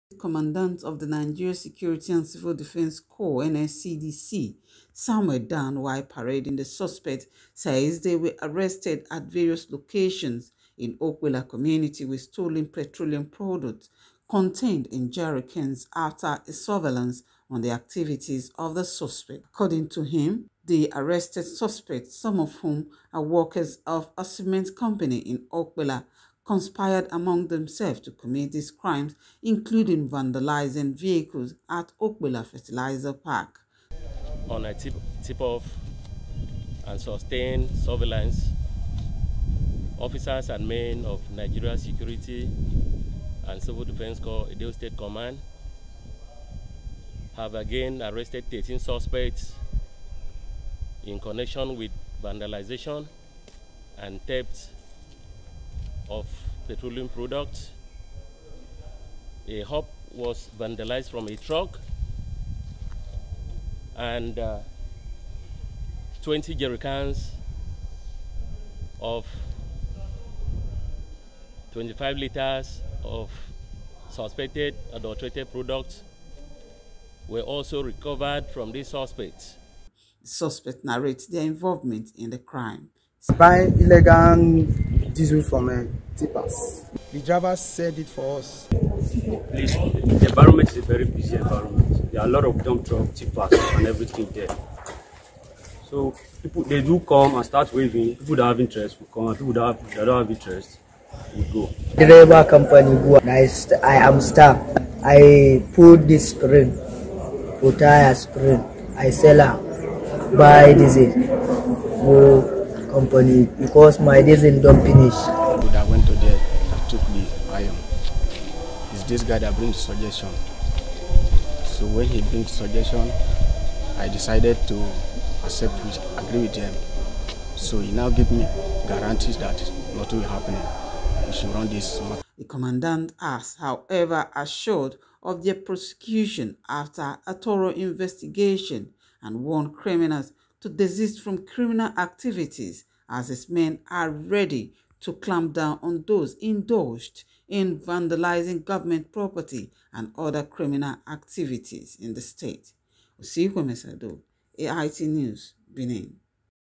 13 SUSPECTS ARRESTED BY NSCDC IN EDO STATE FOR VANDALISM AND THEFT
Thirteen persons arrested by Edo state command of the Nigeria security and civil defence corps for conspiring to steal petroleum products and vehicle spare parts at fertilizer park  in Okpella, Edo state.
The suspects with items stolen were paraded at the command’s headquarters in Benin city, Edo state.